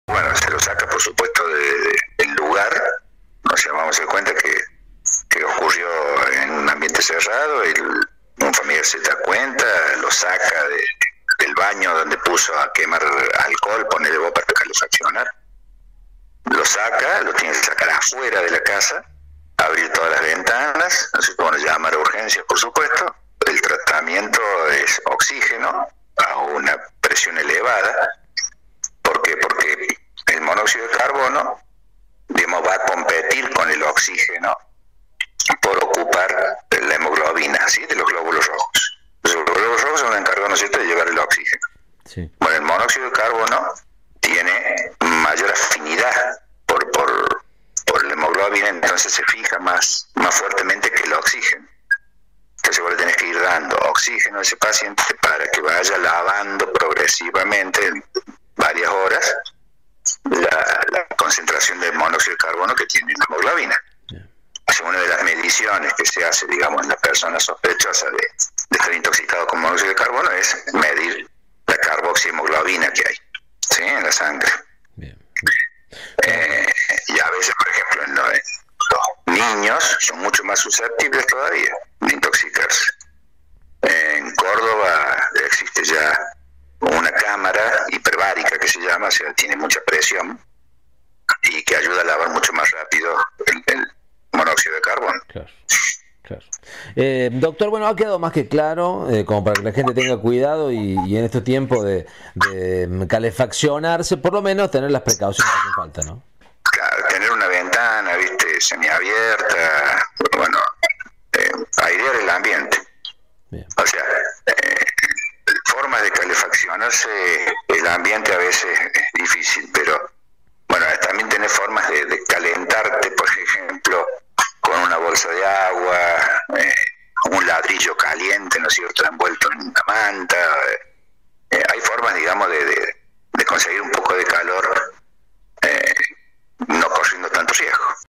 EL MONÓXIDO ES UNA AMENAZA INVISIBLE
Puede causar la muerte en poco tiempo si no se asiste a la persona intoxicada en forma rápida
DR. RAFAEL FONZO - Médico Clínico